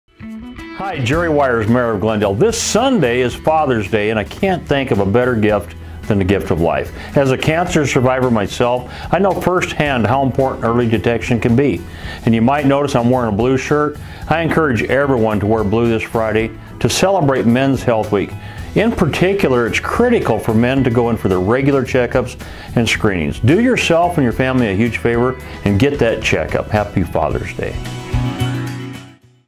Men's Health Week- Glendale
Men's Health Week focuses on men seeking preventative screenings and care. Take control of your health!